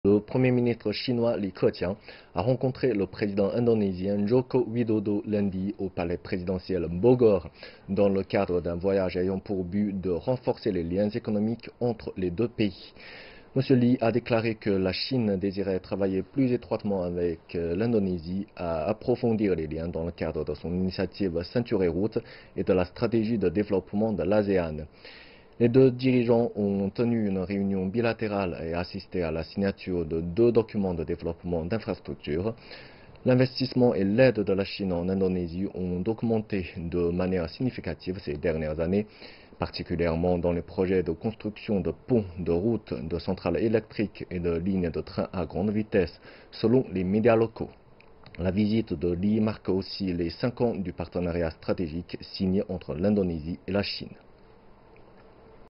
Li Keqiang en visite en Indonésie
Le Premier ministre chinois Li Keqiang a rencontré le Président indonésien Joko Widodo lundi au Palais présidentiel Bogor dans le cadre d'un voyage ayant pour but de renforcer les liens économiques entre les deux pays. M. Li a déclaré que la Chine désirait travailler plus étroitement avec l'Indonésie à approfondir les liens dans le cadre de son initiative Ceinture et Route et de la Stratégie de développement de l'ASEAN. Les deux dirigeants ont tenu une réunion bilatérale et assisté à la signature de deux documents de développement d'infrastructures.